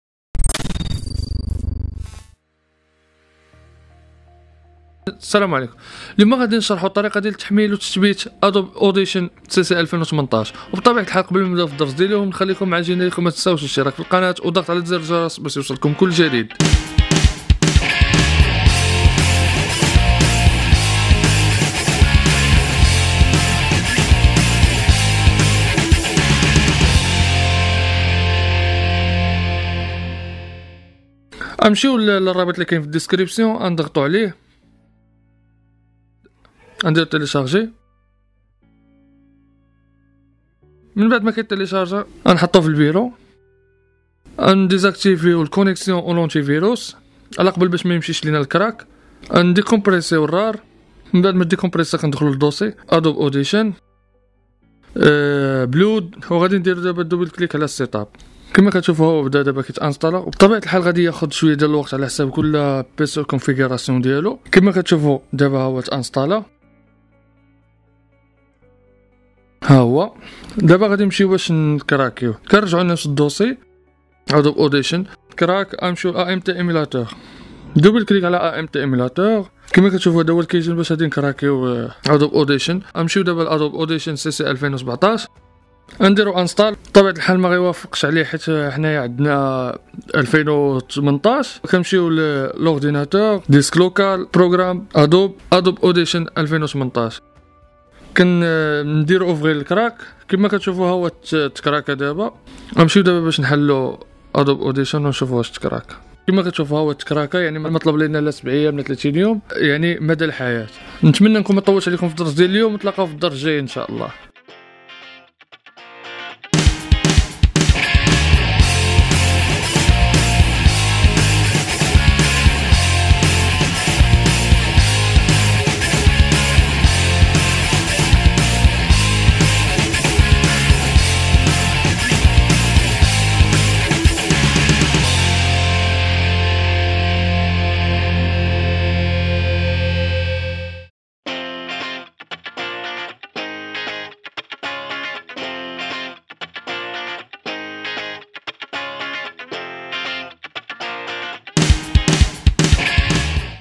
طريقة تحميل وتثبيت برنامج [Adobe Audition CC 2018]
رابط تحميل برنامج_Adobe Audition CC CC 2018

Presented:
Mehdi Boujâada :

how to install adobe Audition cc